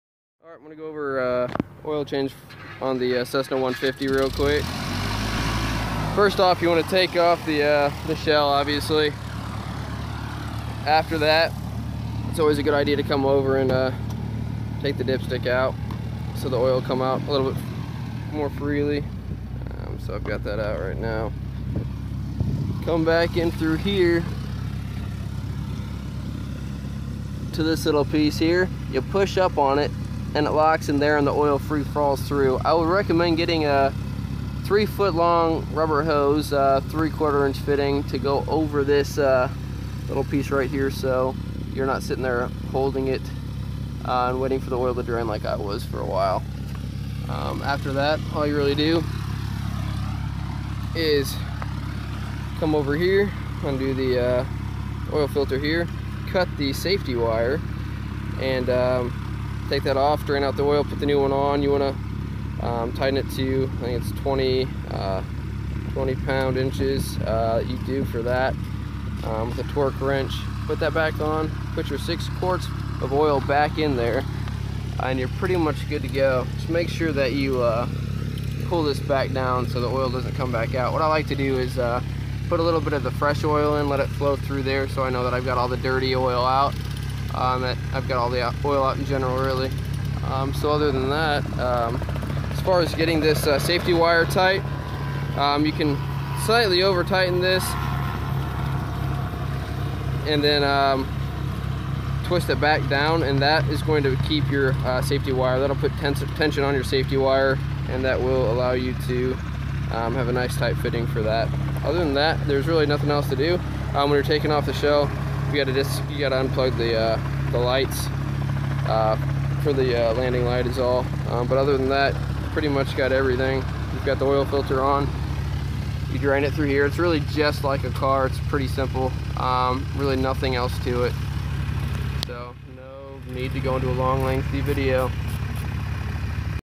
Cessna 150 0-200 oil change
Quick explanation on how to do an oil change on a cessna 150